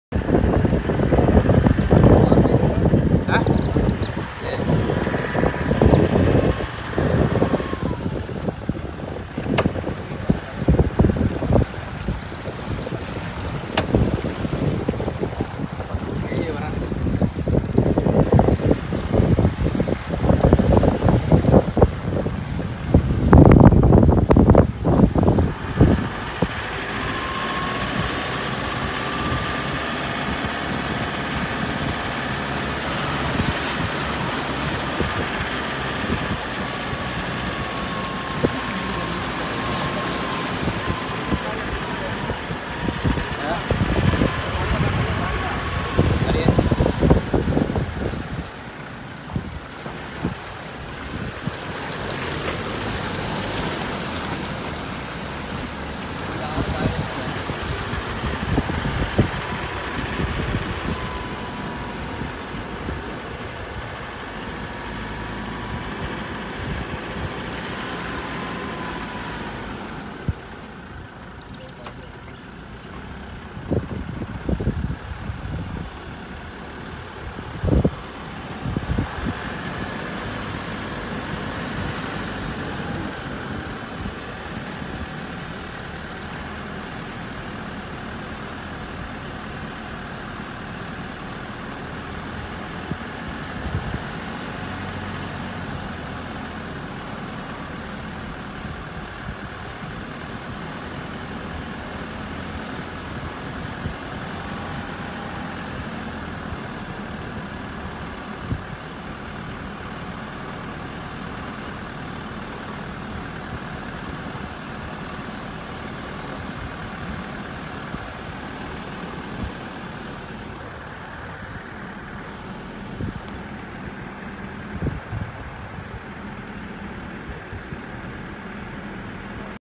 Follow the Giant
Off Road Journey to the KUMANA National Park 2018 with Defender PUMA